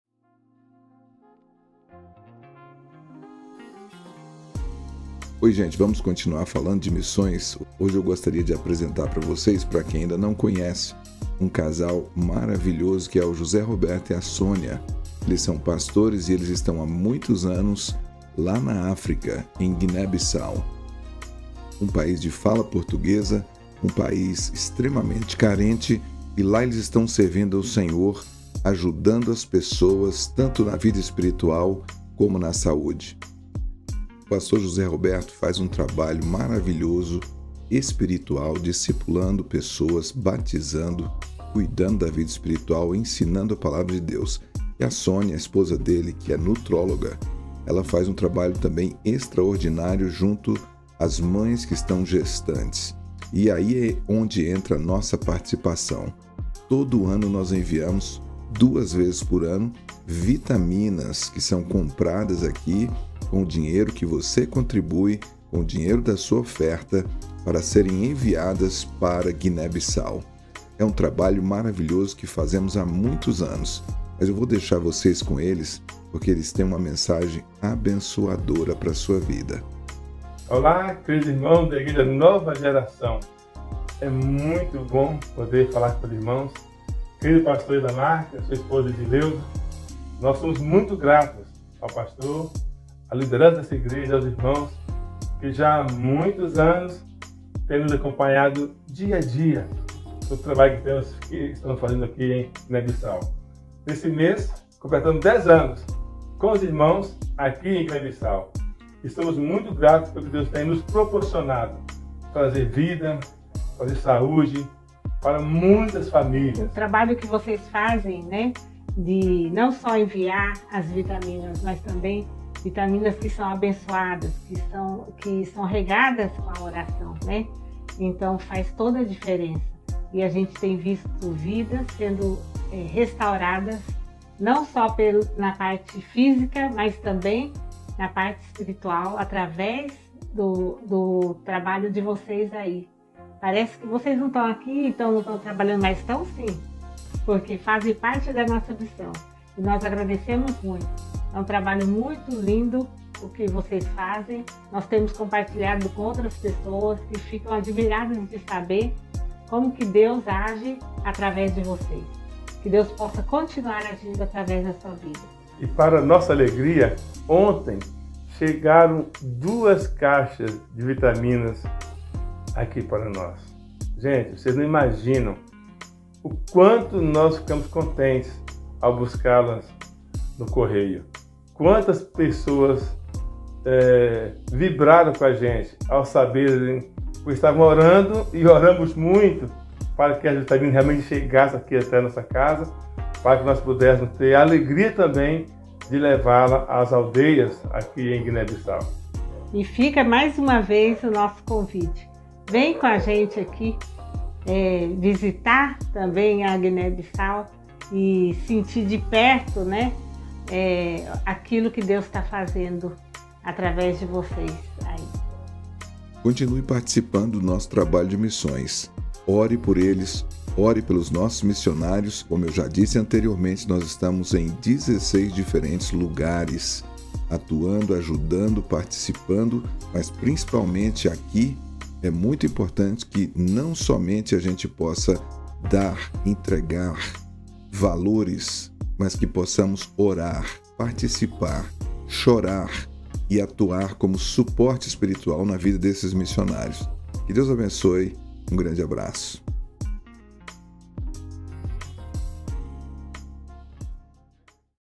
Missões NG Denver - Guine Bissau
Agradecimento do Pr J Roberto e Sonia sobre a participação da New Generation Denver com o envia de Vitaminas.